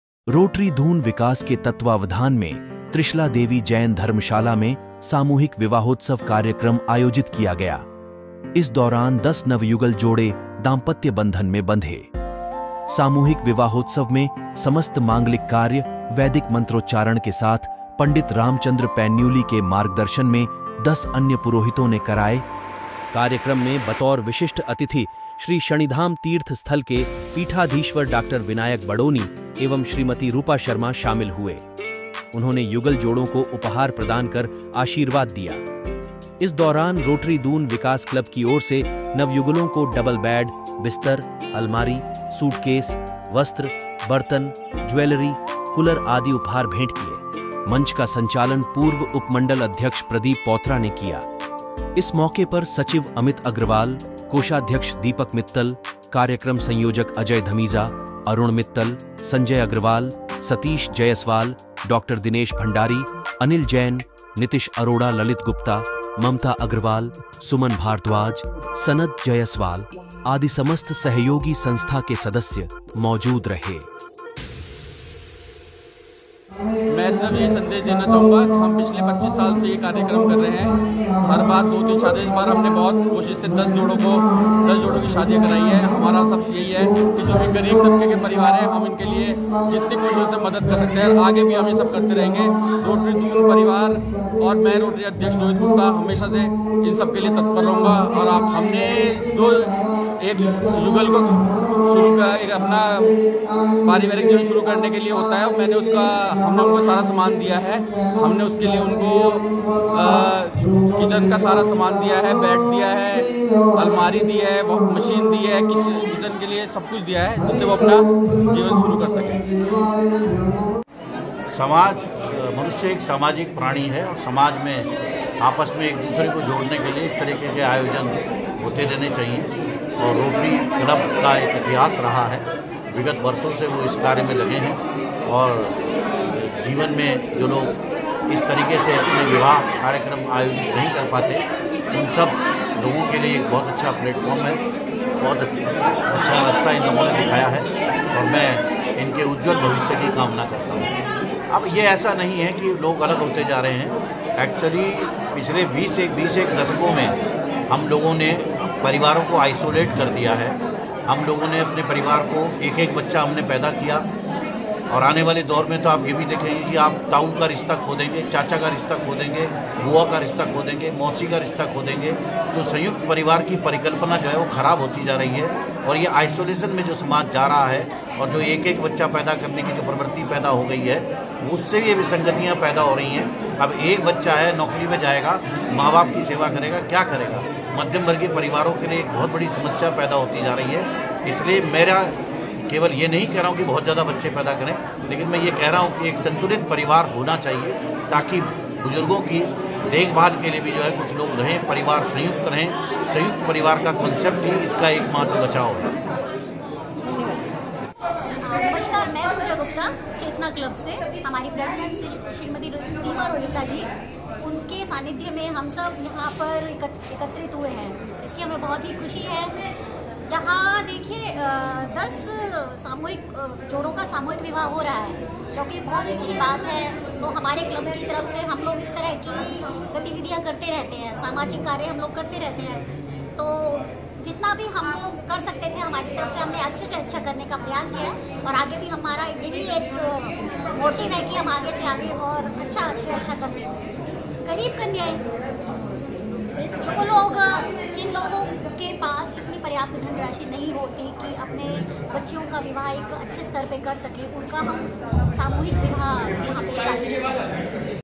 रोटरी दून विकास के द्वारा सामूहिक विवाहोत्सव कार्यक्रम का आयोजन।
रोटरी दून विकास के तत्वावधान में त्रिशला देवी जैन धर्मशाला में सामूहिक विवाहोत्सव कार्यक्रम आयोजित किया गया। इस दौरान दस नवयुगल जोडे दांपत्य बंधन में बंधे। सामूहिक विवाहोत्सव में समस्त मांगलिक कार्य वैदिक मंत्रोच्चारण के साथ पं. रामचंद्र पैन्यूली के मार्गदर्शन में दस अन्य पुरोहितों ने कराया कार्यक्रम में बतौर विशिष्ट अतिथि श्री शनिधाम तीर्थ स्थल के पीठाधीश्वर डाक्टर विनायक बडोनी शामिल हुए। उन्होंने युगल जोड़ों को उपहार प्रदान कर आशीर्वाद दिया। इस दौरान रोटरी दून विकास क्लब की ओर से नवयुगलों को डबल बैड, बिस्तर, अलमारी, सूटकेस, वस्त्र, बर्तन, ज्वेलरी, कुलर आदि उपहार भेंट किए। मंच का संचालन पूर्व उप मंडल अध्यक्ष प्रदीप पौत्रा ने किया इस मौके पर सचिव अमित अग्रवाल, कोषाध्यक्ष दीपक मित्तल, कार्यक्रम संयोजक अजय धमीजा, अरुण मित्तल, संजय अग्रवाल, सतीश जयसवाल, डॉक्टर दिनेश भण्डारी, अनिल जैन, नितिश अरोड़ा ललित गुप्ता,  ममता अग्रवाल, सुमन भारद्वाज, सनद जयसवाल आदि समस्त सहयोगी संस्था के सदस्य मौजूद रहे।